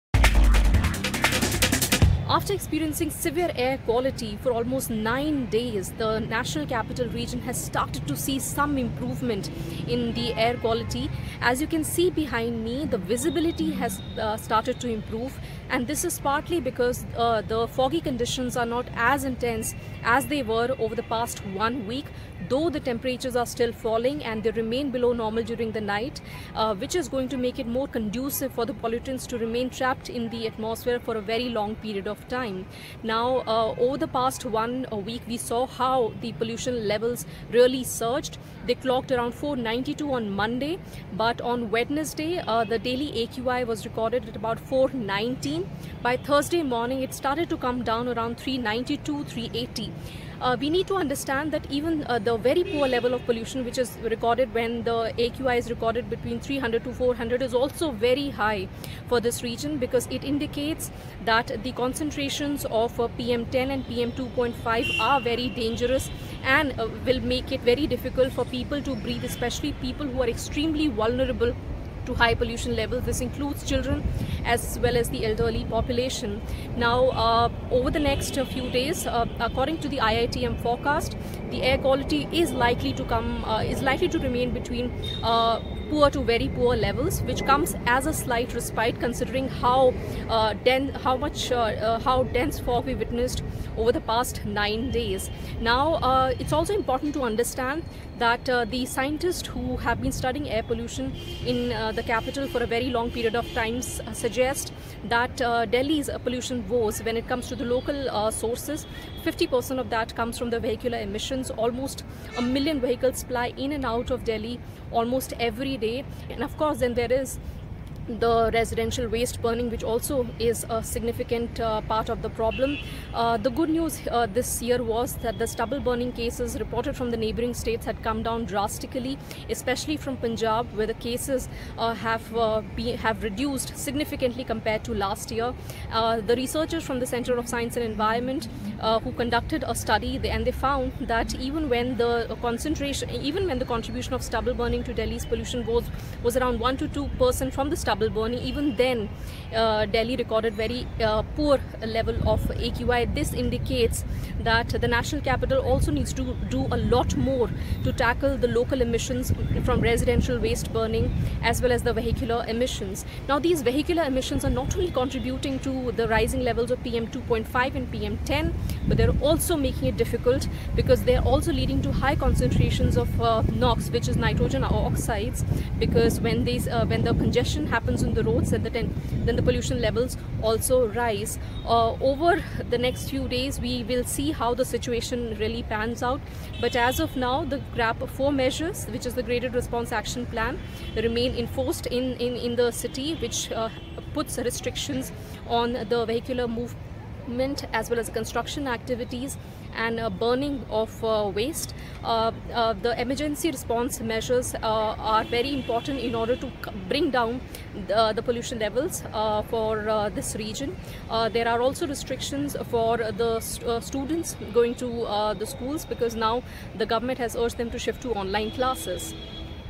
Delhi Pollution | Air quality Improves To 'Very Poor' Category in Delhi | Delhi AQI | N18V | News18
Though the overall air quality has improved marginally, many places in Delhi continue to reel under ‘severe’ air pollution on Thursday (November 21, 2024).

After staying in the ‘severe’ and ‘severe plus’ category for several days, the overall air quality of Delhi improved slightly to the ‘very poor’ level on Thursday (November 21, 2024) morning, according to official data.